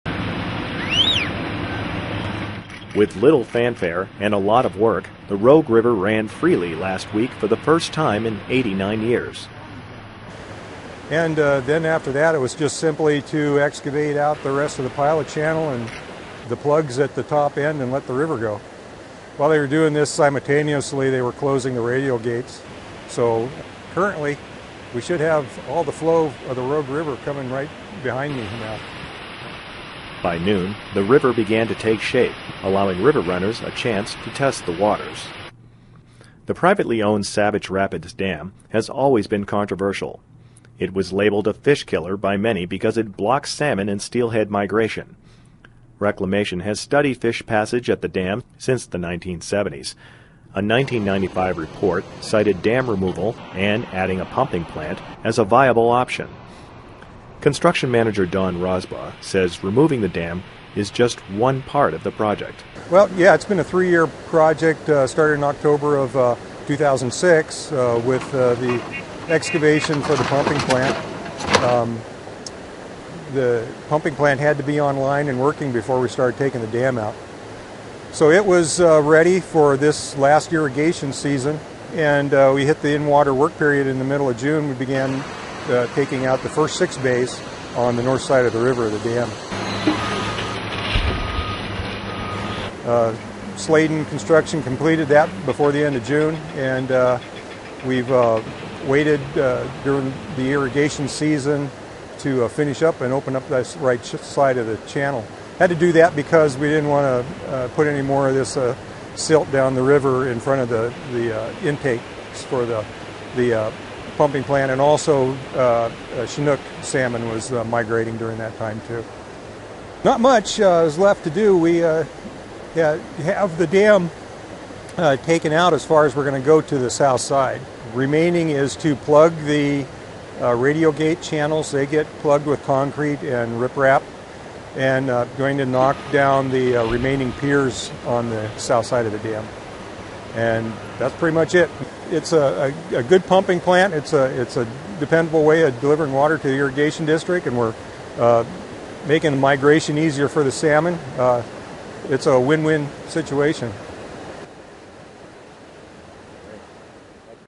A River Runs Through It: The Removal of Savage Rapids Dam
A River Runs Through It: The Removal of Savage Rapids Dam - On October 9, 2009, the last remaining radial gates at Savage Rapids Dam were closed, returning the Rogue River to its historic stream channel for the first time since 1921 and allowing salmon and trout the opportunity to migrate freely to and from the Pacific Ocean. A pumping plant was built downstream of the dam site to meet irrigation needs in the area. The total project cost was $39.3 million.